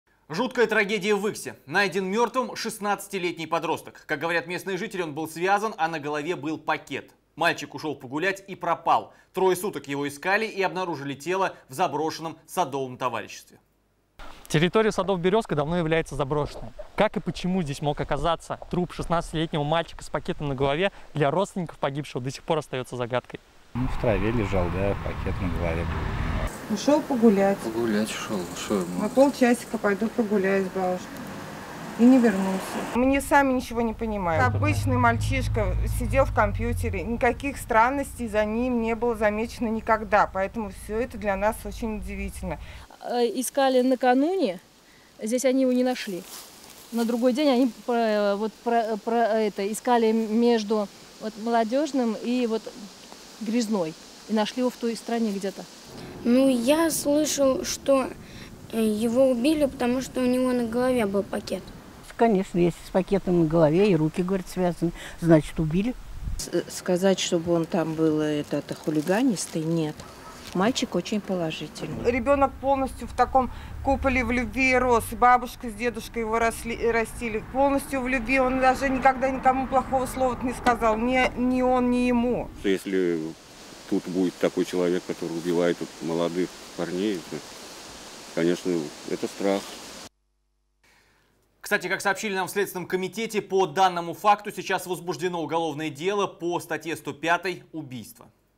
Мертвый подросток обнаружен связанным с пакетом на голове
Жуткая трагедия в Выксе - найден мертвым 16-летний подросток. Как говорят местные жители - он был связан, а на голове был пакет. Мальчик ушел погулять и пропал. 3 суток его искали и обнаружили тело в заброшенном садовом товариществе.